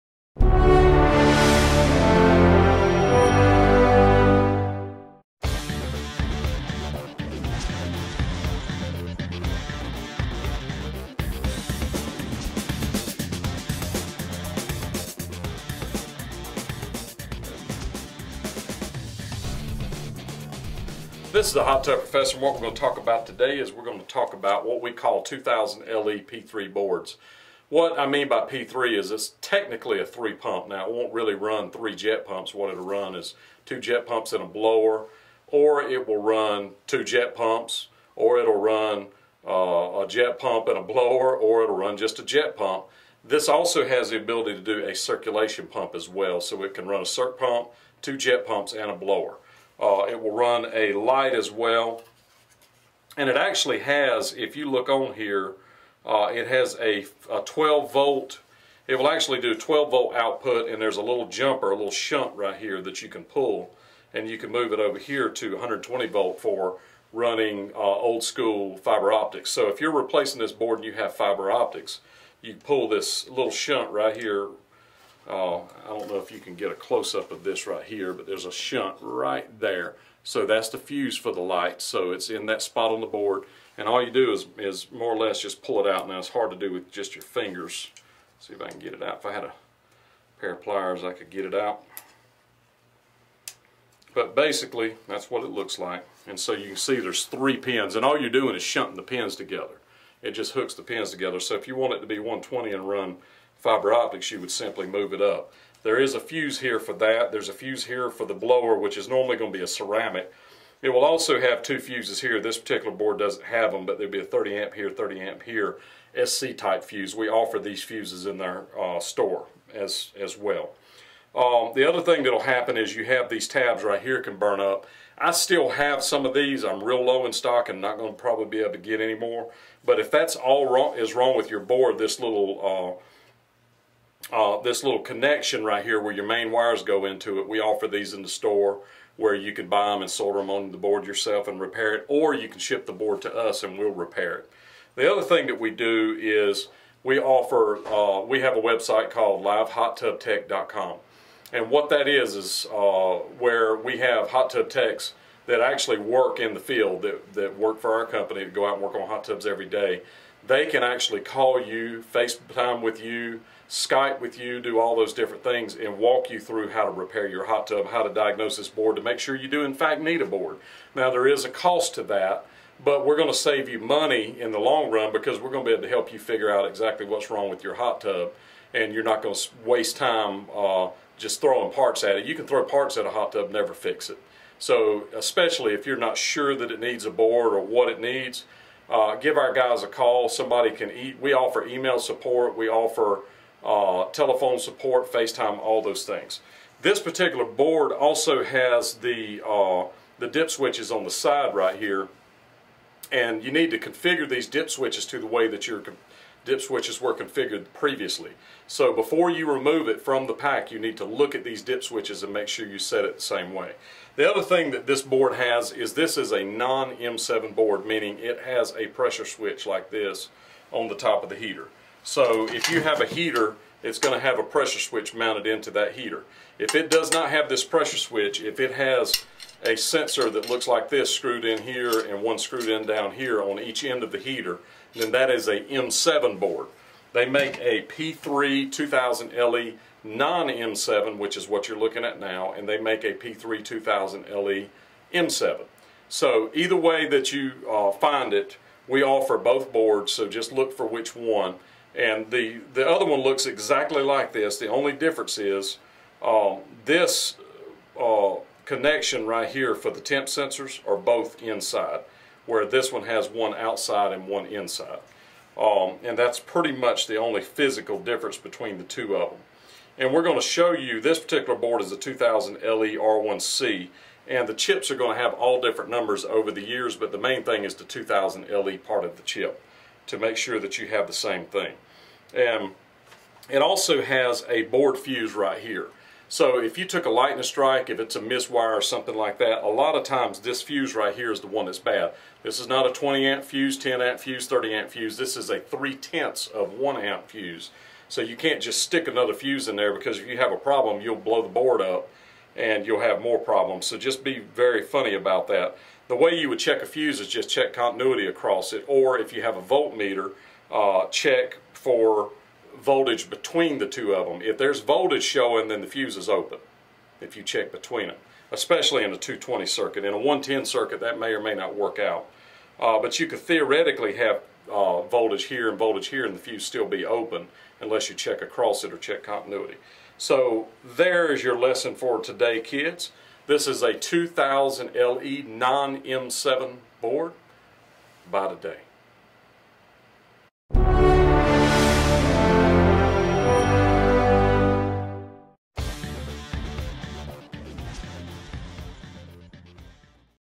Hot Tub Professor Balboa 2000LE P3 Board How To
The Hot Tub Professor talks about a Balboa 2000LE P3 Board. For technical help go to the buttons on the top right of the video for parts and also Live Hot Tub Technical Help..